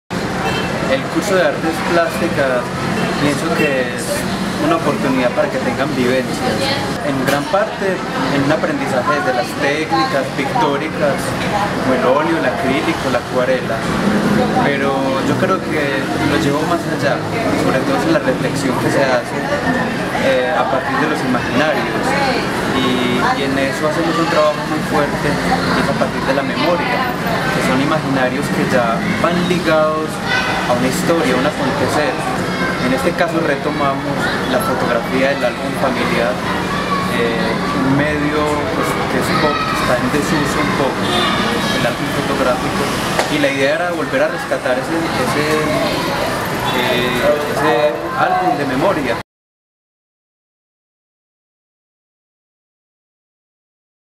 Dubian Monsalve - Docente Formación Artística
Muestras de Formación Artística y Cultural del Centro de Desarrollo Cultural de Moravia 2018-1